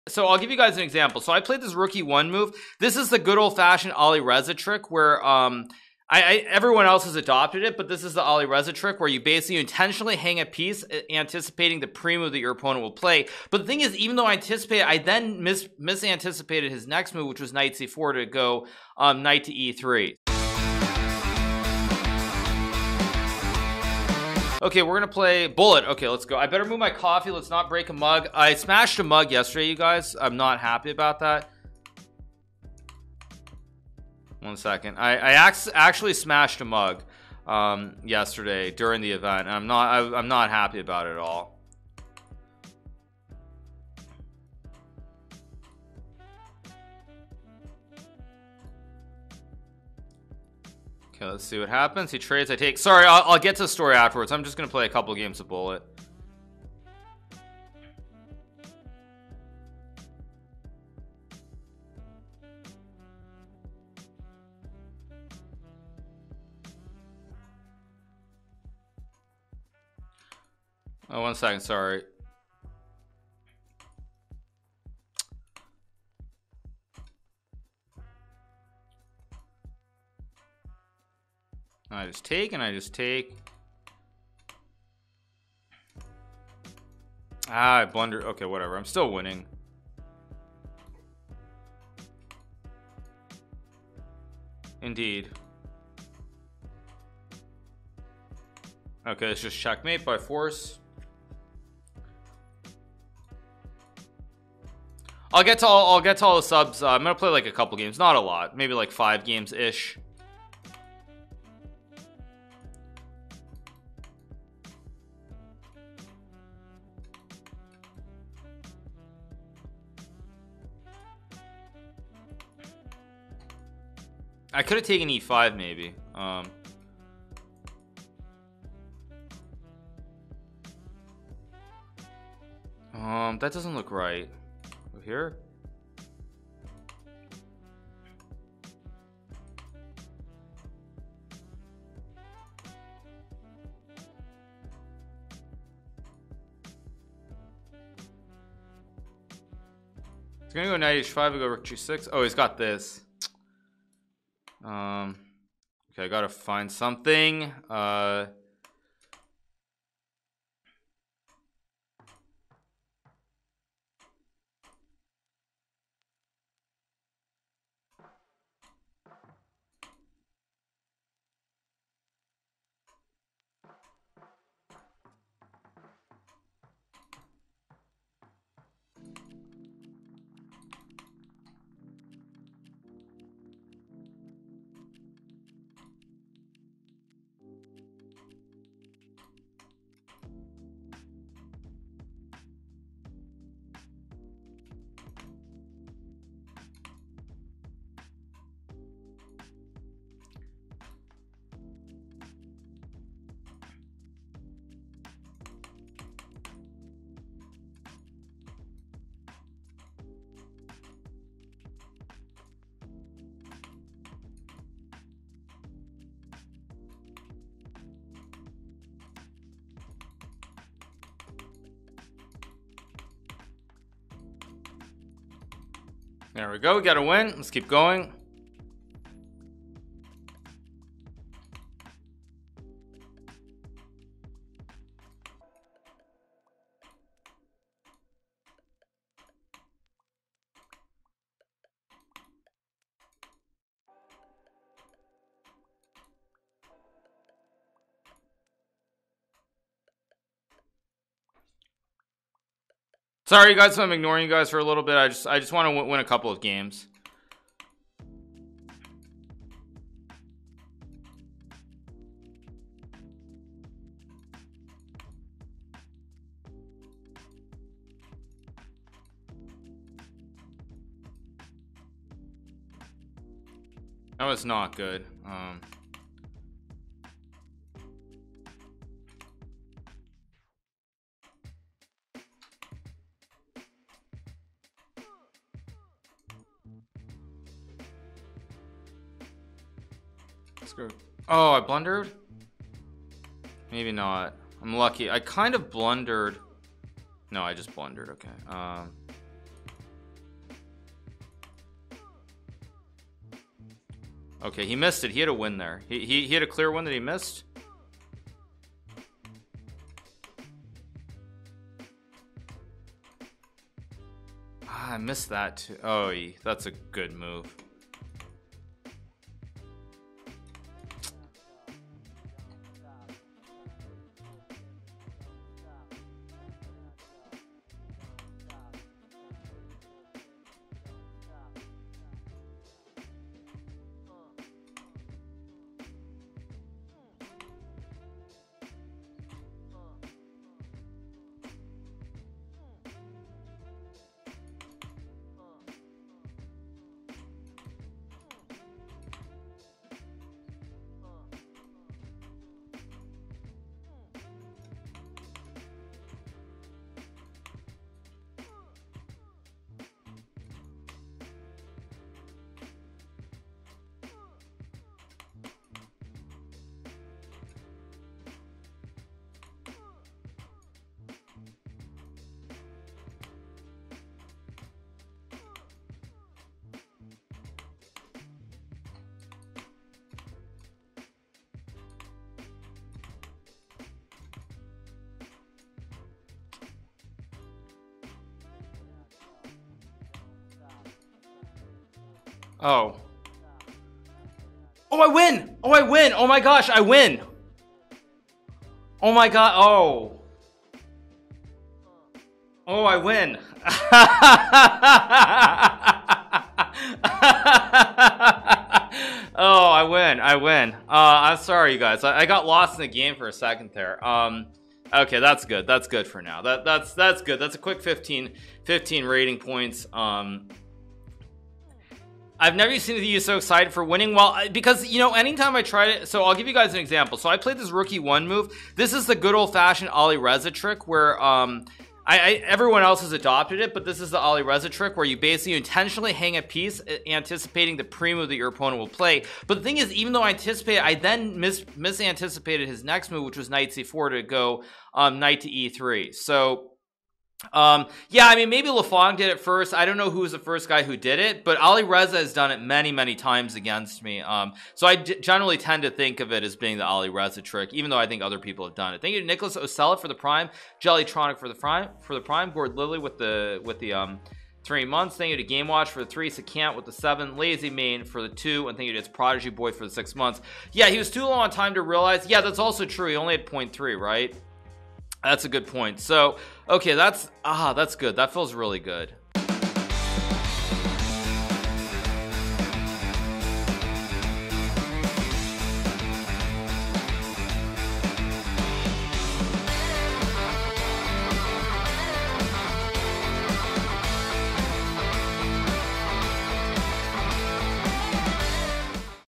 How to Win at Bullet Chess: The Alireza Trick
Want to get good at bullet chess? This isn't the video for you. If you want to swindle your way to victory, Hikaru shows you exactly how to do that and exactly how not to do it at the same time.

🌟 Socials 🌟

Join our Reddit for the MEMES and to make it to Reddit React Videos ➡️